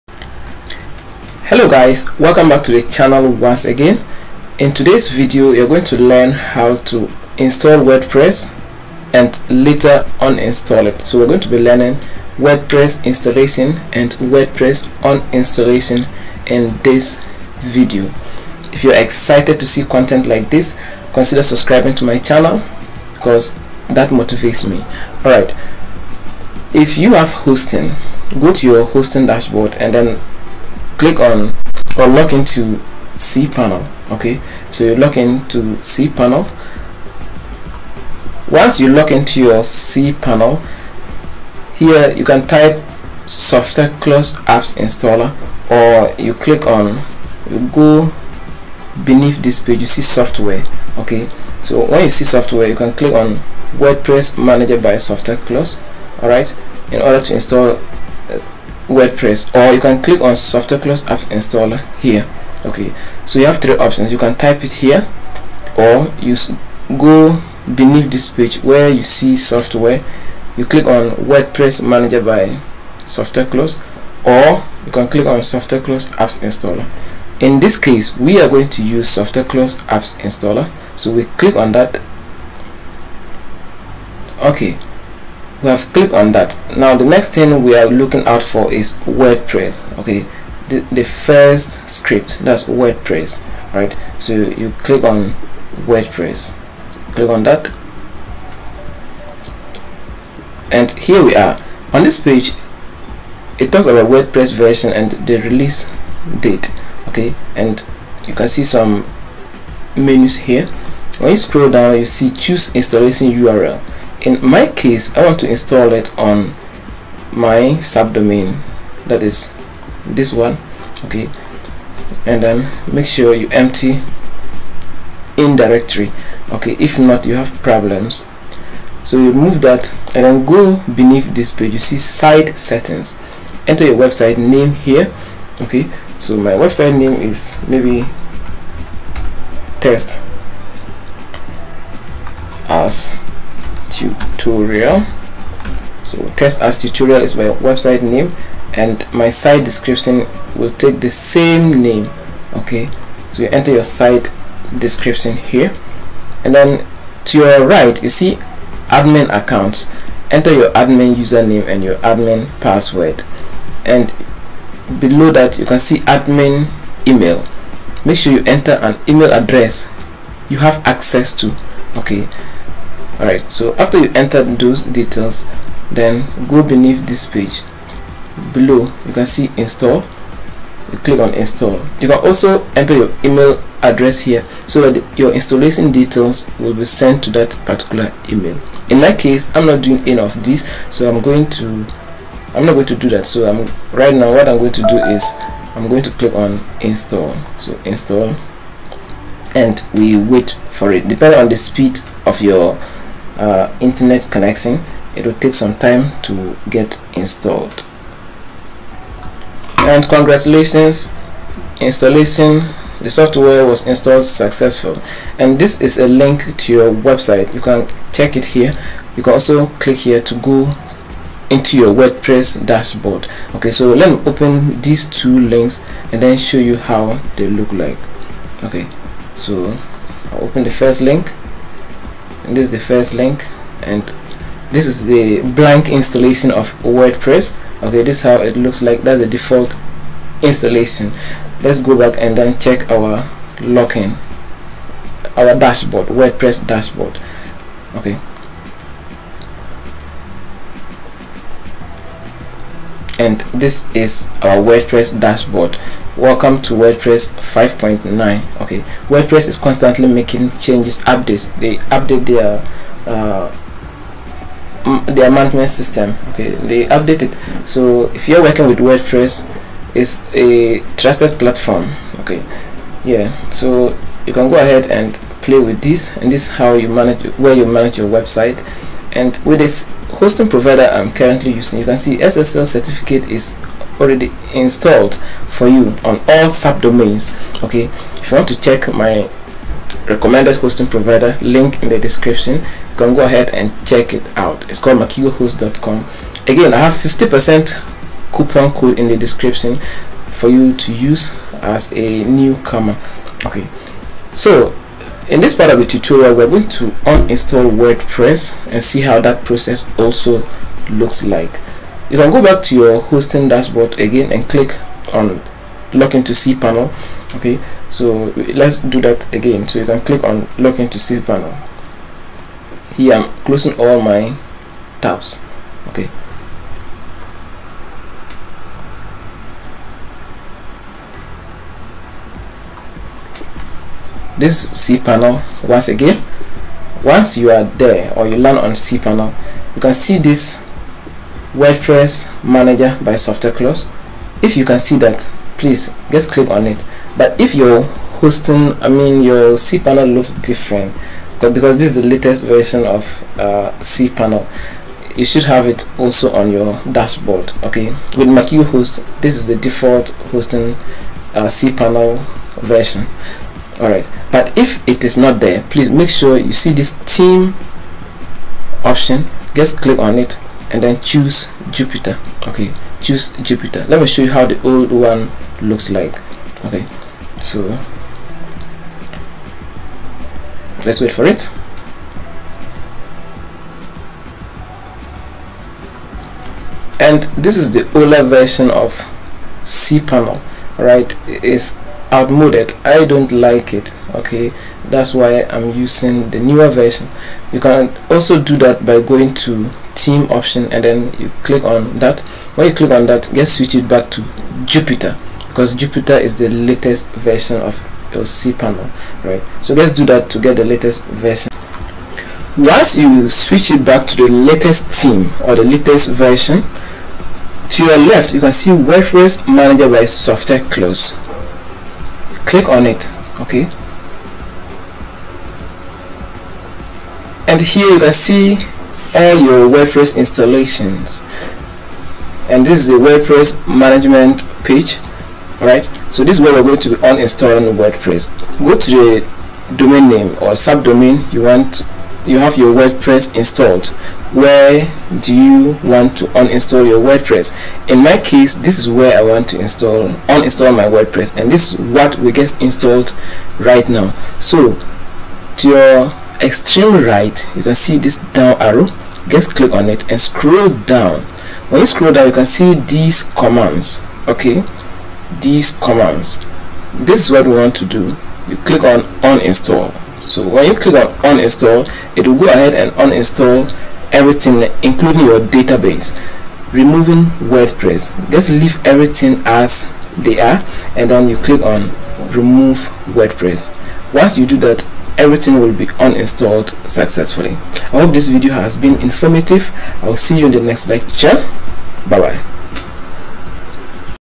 How to install and uninstall wordpress in cPanel
In this video you are going to learn how to install and uninstall wordpress